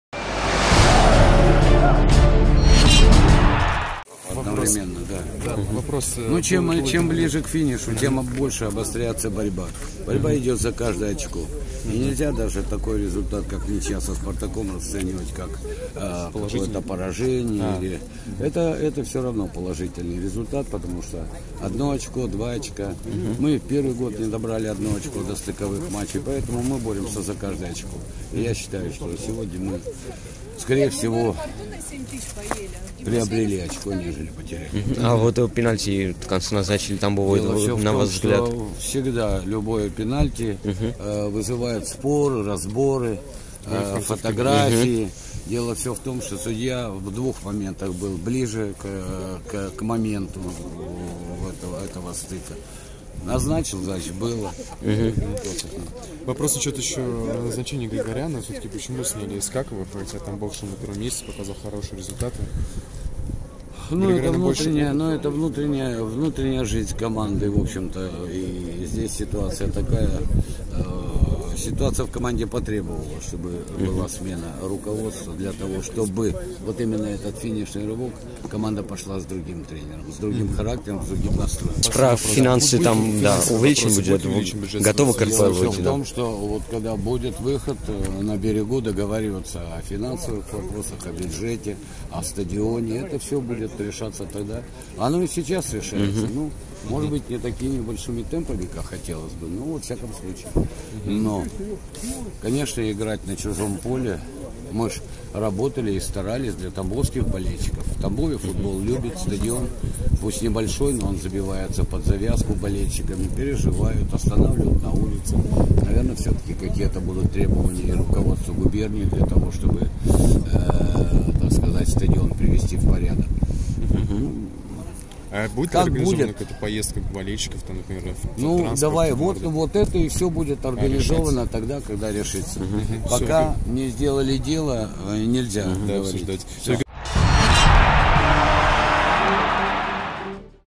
Георгий Ярцев: "Почему сменили Искакова на Григоряна? Мы посчитали, что команде перемены"
Вице-президент "Тамбова" после матча со "Спартаком-2" рассказал, зачем команде понадобилась смена главного тренера, и хватит ли клубу денег для выступления в РПЛ.